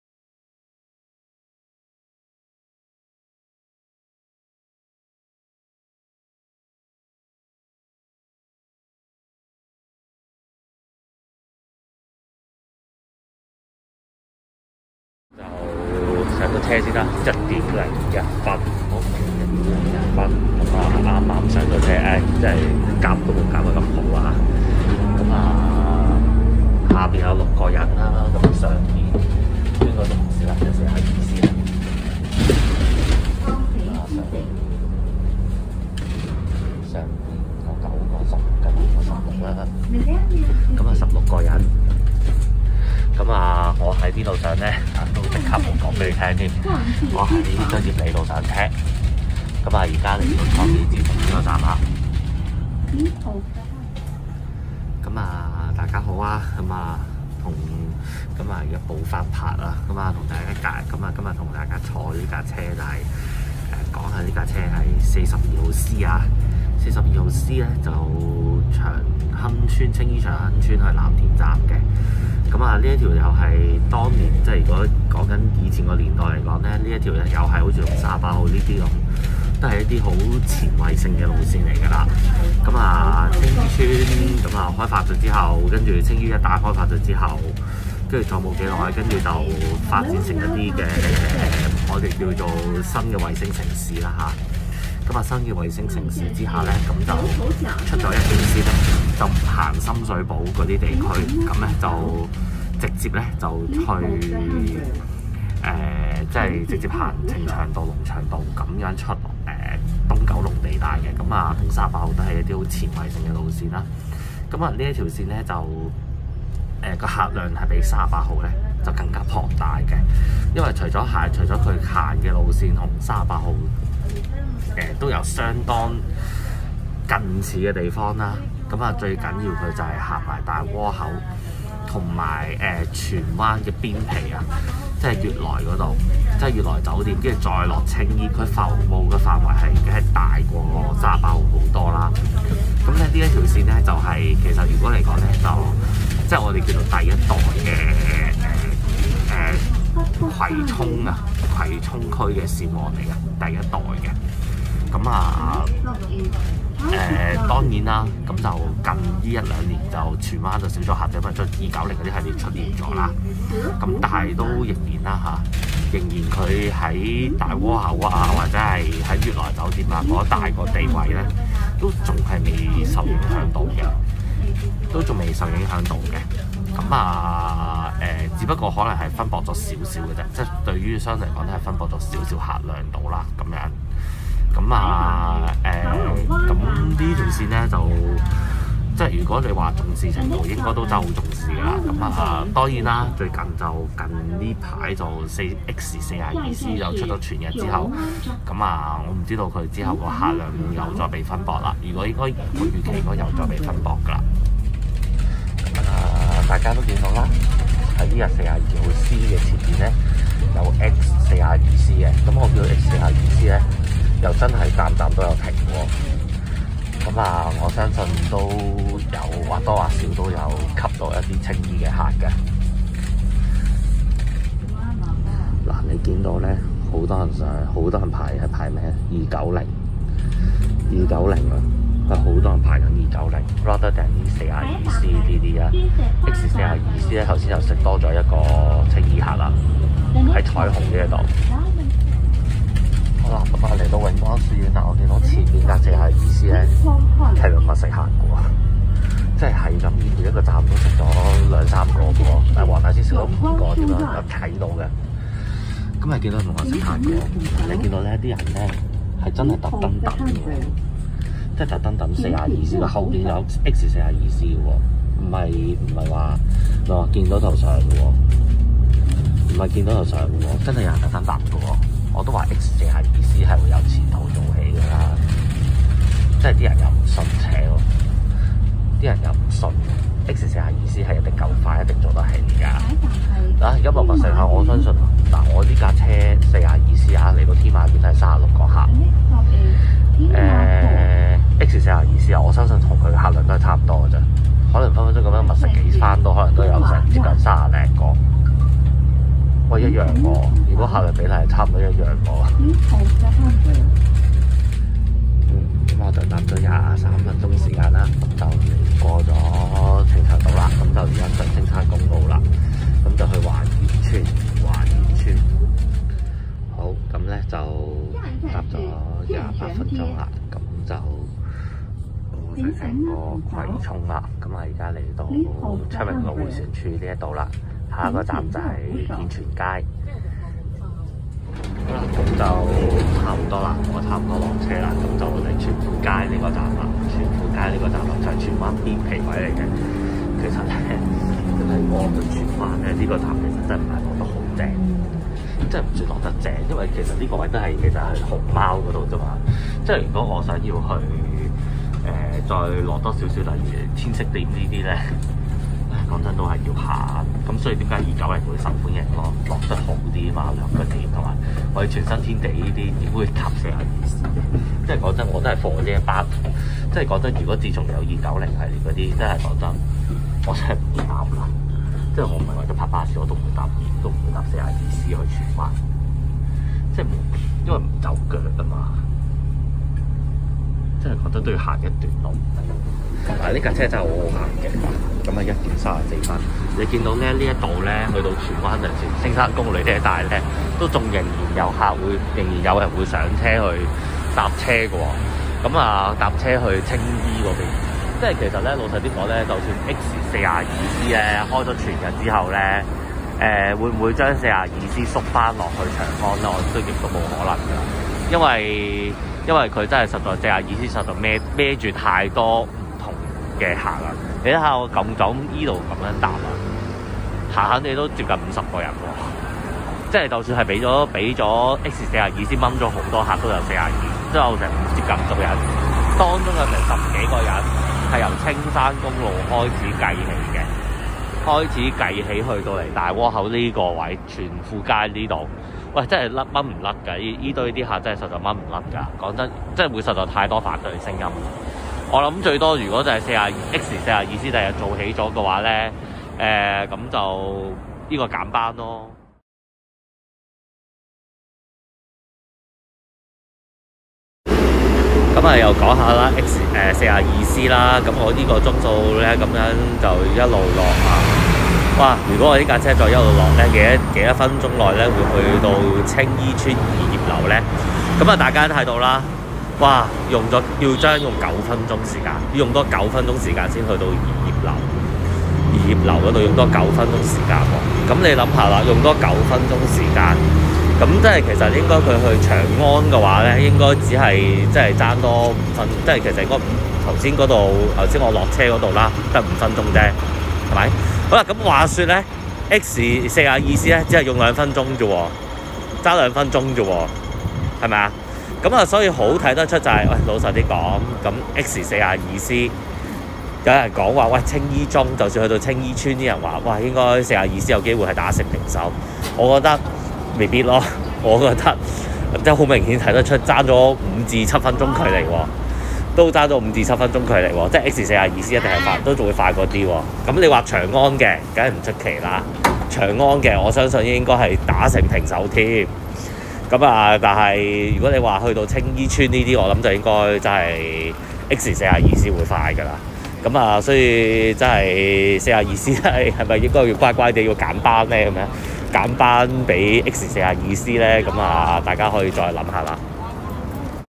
巴士路線評價：九巴42C暨X42C同場比較
藍田站往來青衣（長亨邨）

一睹線王的風采
雖然近年不斷有290系列搶客，惟客量仍可觀
X42C亦漸漸地剝弱42C於青衣中南部地位？

那到底42C能否縮短？
我答案係不能。
只要多留意每一個站上上落落，你便知道。

亦有X42C於觀塘開始既同場前後腳比較